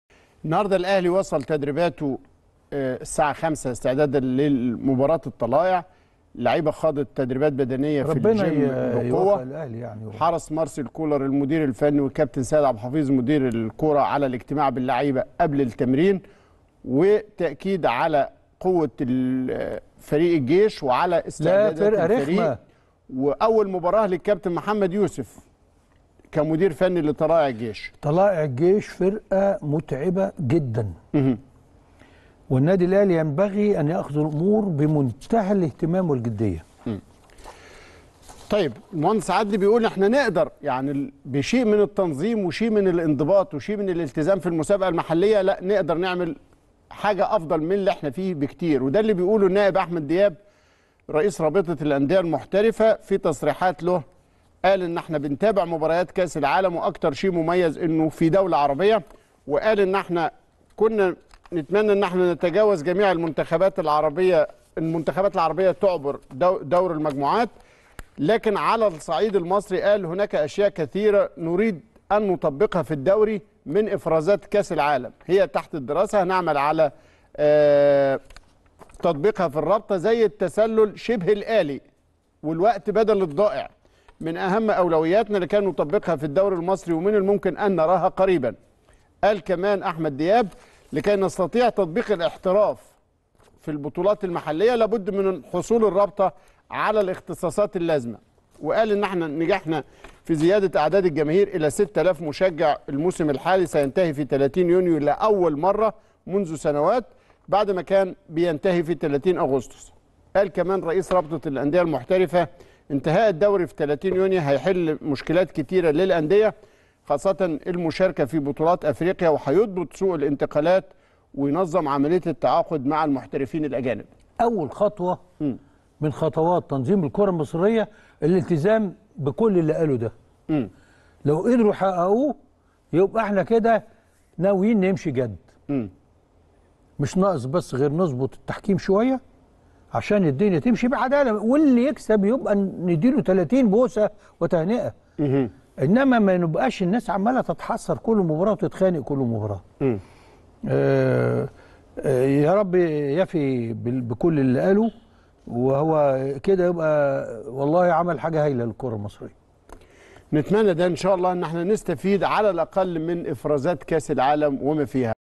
عدلي القيعي: طلائع الجيش فرقه متعبة جدا والنادي الأهلي ينبغي أن يأخد الأمور بمنتهي الأهتمام والجدية
عدلي القيعي: طلائع الجيش فرقه متعبة جدا والنادي الأهلي ينبغي أن يأخد الأمور بمنتهي الأهتمام والجدية | ملك وكتابة